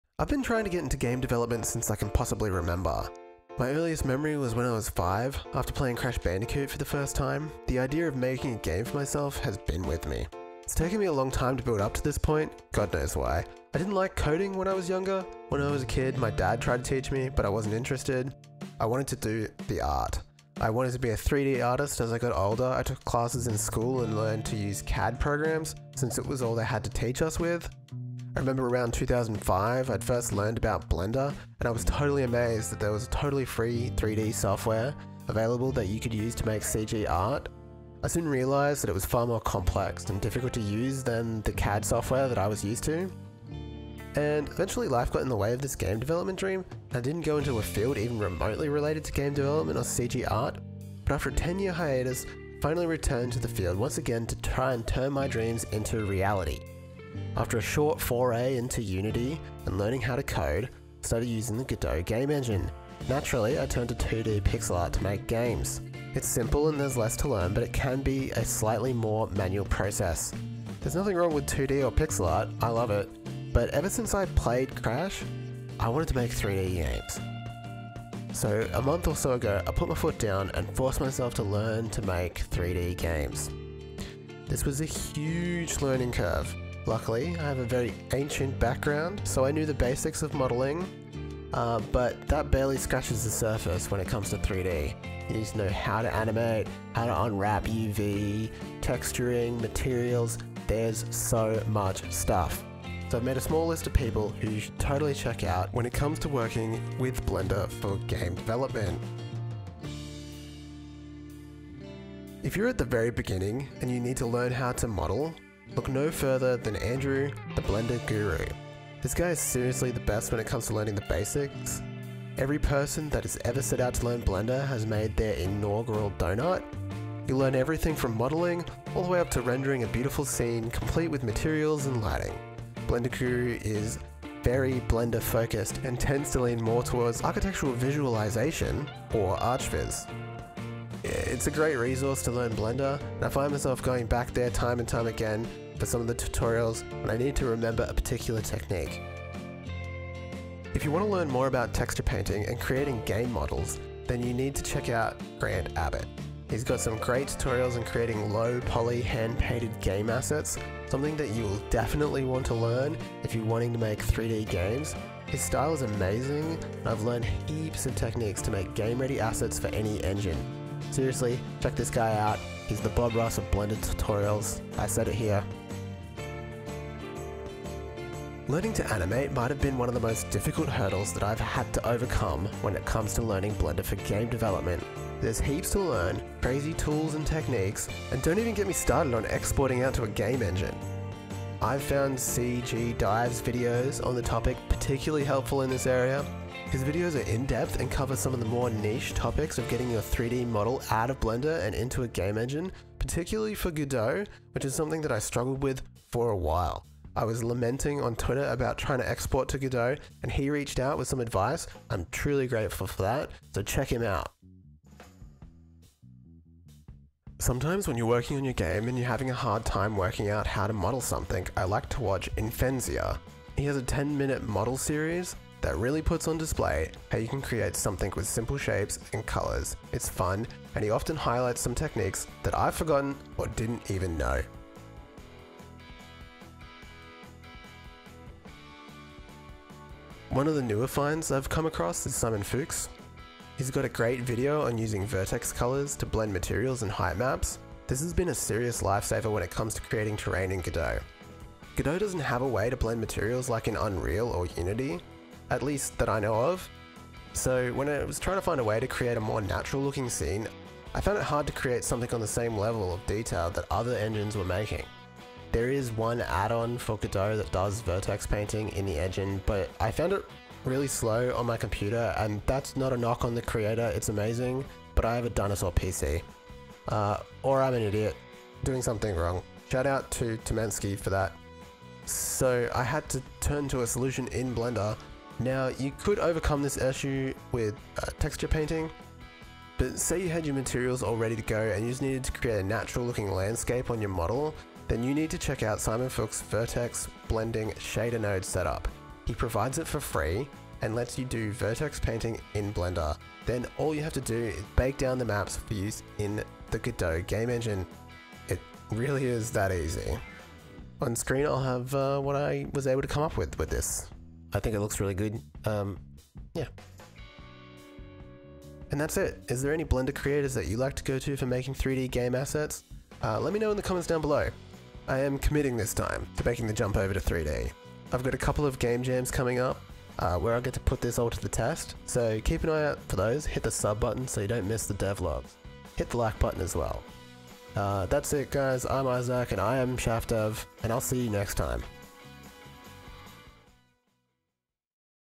5 YouTubers that helped me get started with Blender for Game Dev.
Recently I've been trying to make the jump to 3d when making games. This has been no easy feat. And I have had the fortune of learning from some really amazing people. I've made a list of some of the channels that have given me the skills to make 3d models and export them from Blender into the Godot Game Engine. Learning something new is always a challenge so I thought I would bring together the people that I have been using to get me to a better level. Maybe one day I will also be making Blender tutorials... We'll See.